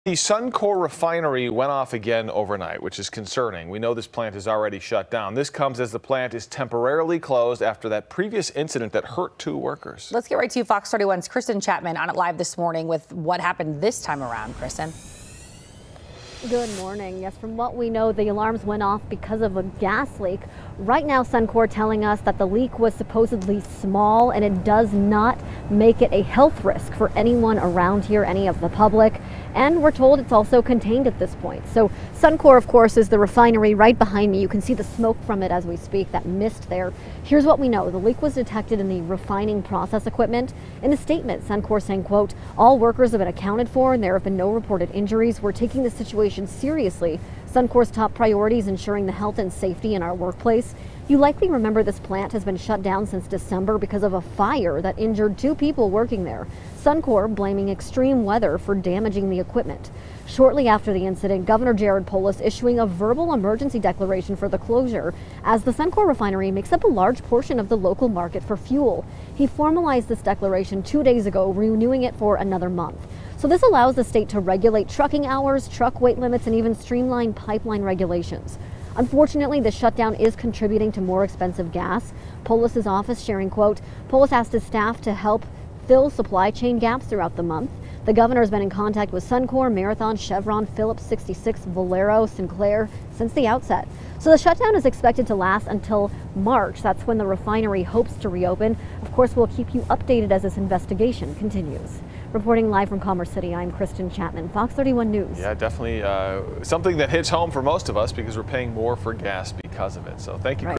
Suncor refinery leaks gas, Polis renews shutdown emergency
Suncor refinery leaks gas, Polis renews shutdown emergency, Kristen Chapman reports